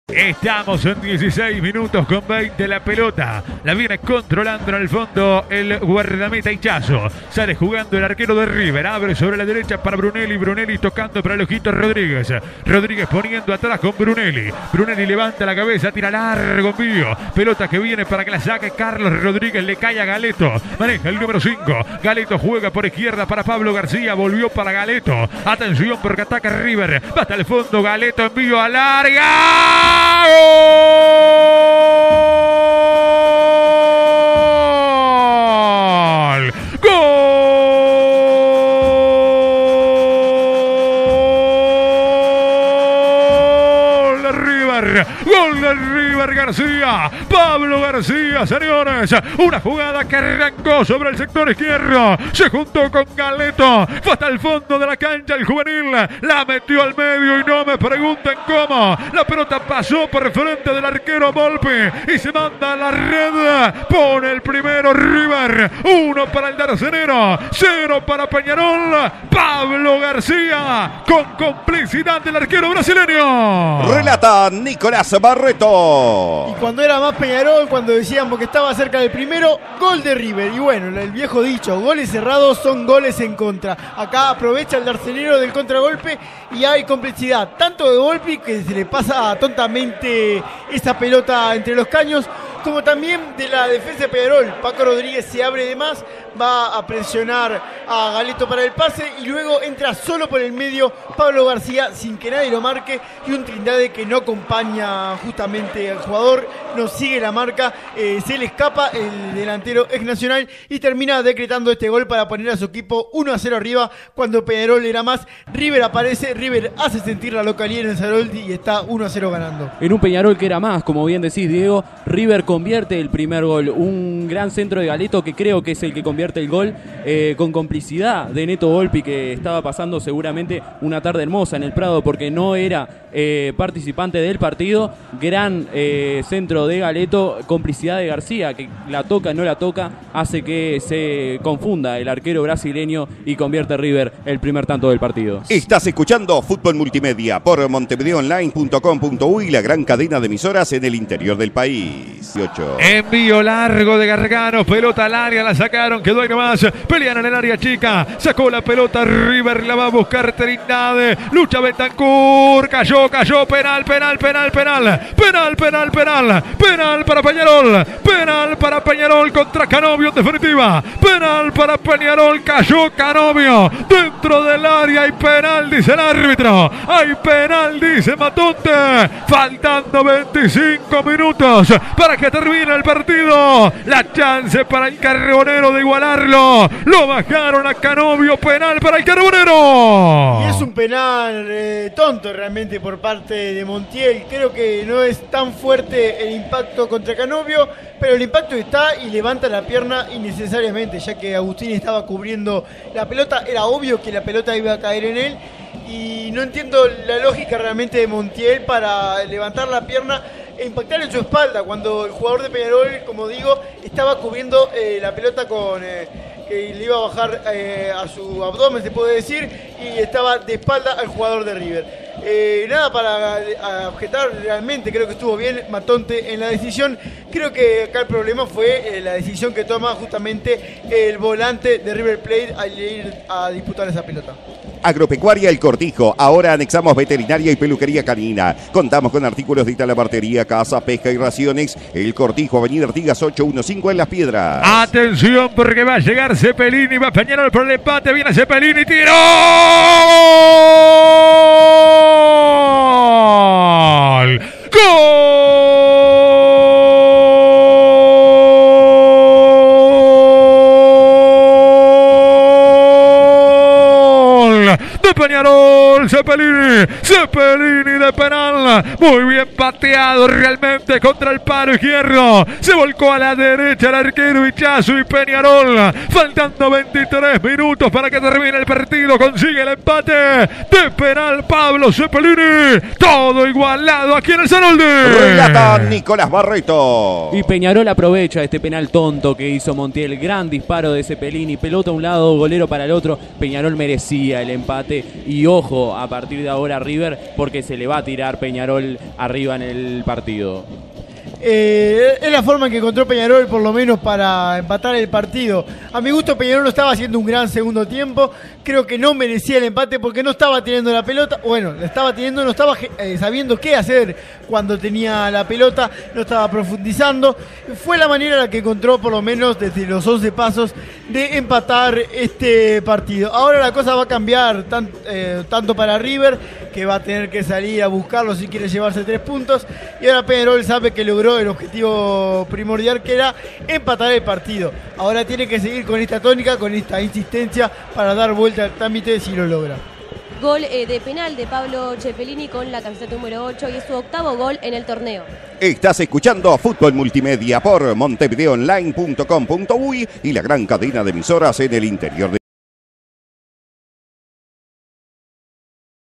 River Plate 1 Peñarol 1 - Clausura 2021 (por Nicolás Barreto)
Los goles del partido, por Nicolás Barreto y el equipo de Fútbol Multimedia para Montevideo Online.